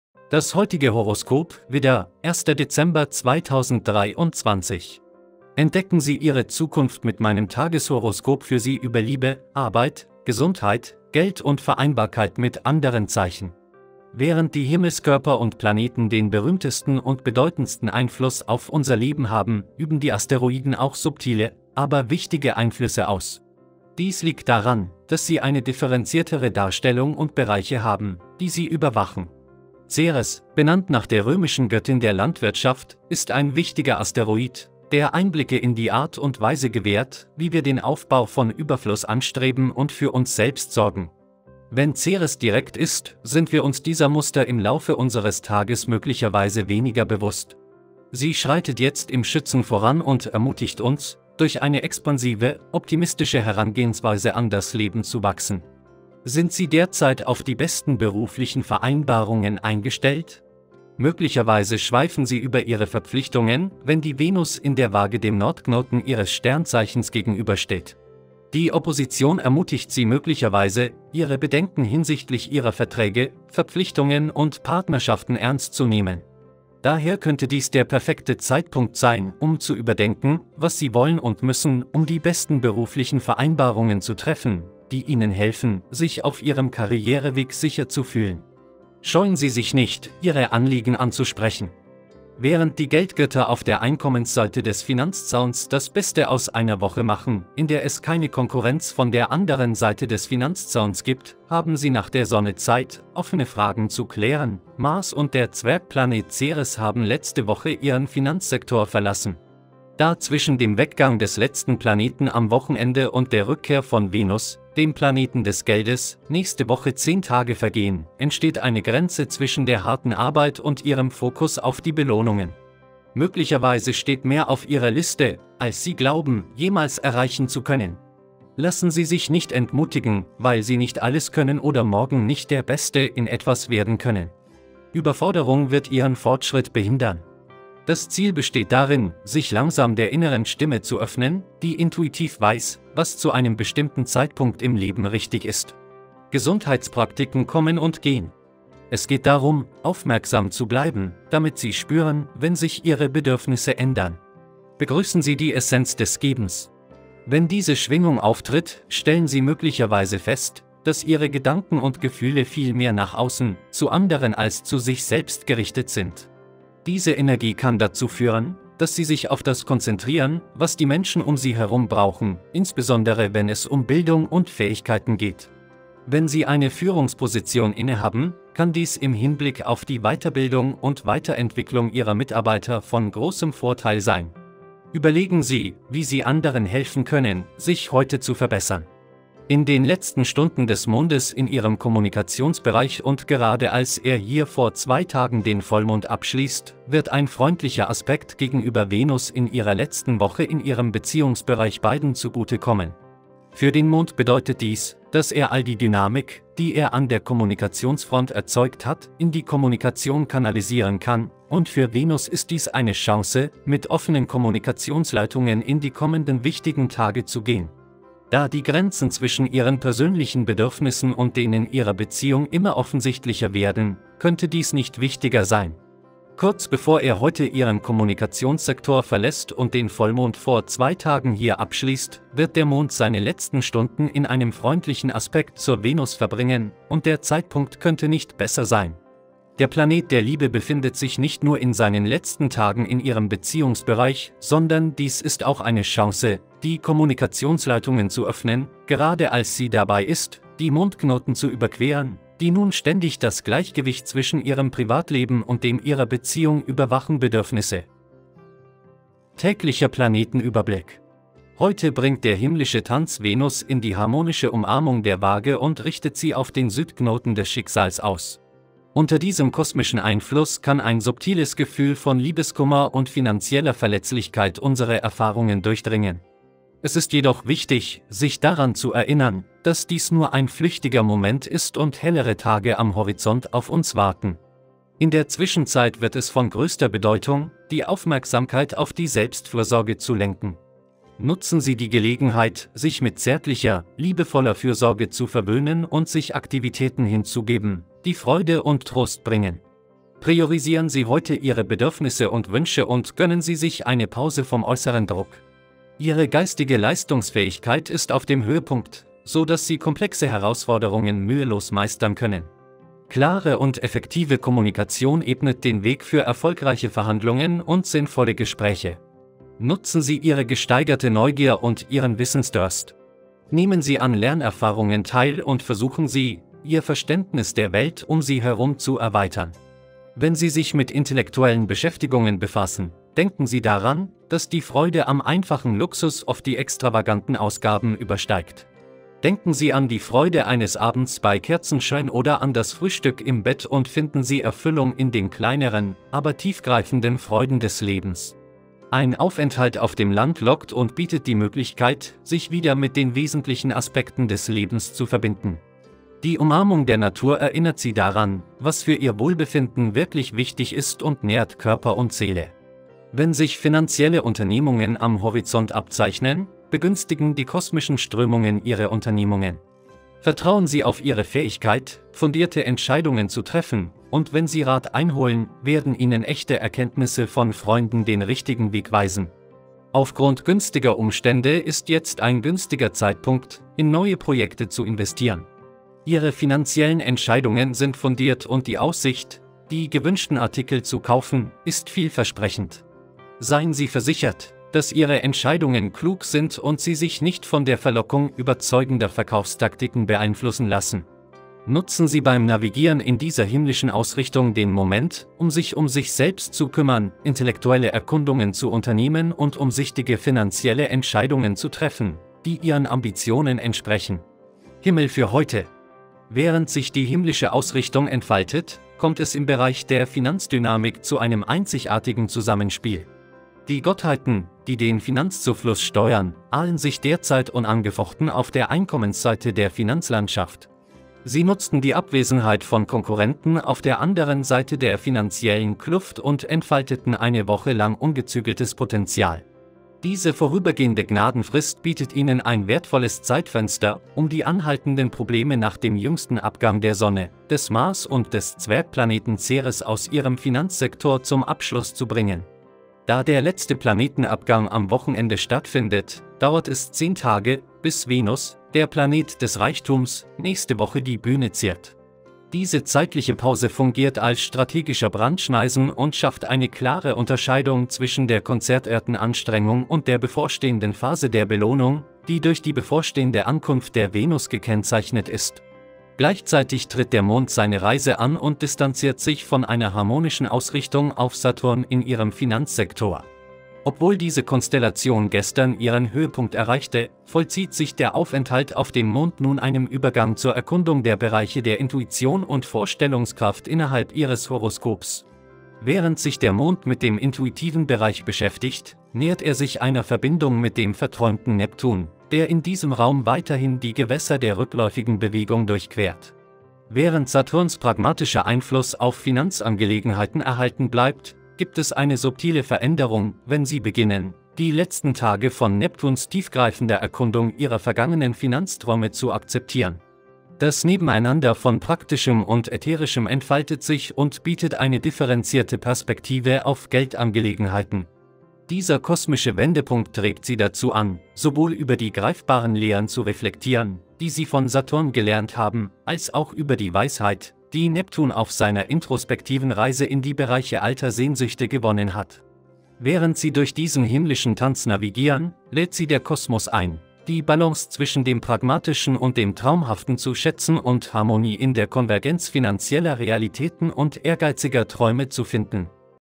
ICH ZIEHE DEN HUT VOR DIR 🚨🤠👏🏻 DU WIRST BENEIDET 🚷 ARIES ♈ HEUTE, 1. DEZEMBER 2023 🔮
Das heutige Horoskop, 1. Dezember 2023. Entdecken Sie Ihre Zukunft mit meinem Tageshoroskop für Sie über Liebe, Arbeit, Gesundheit, Geld und die Verträglichkeit mit anderen Zeichen.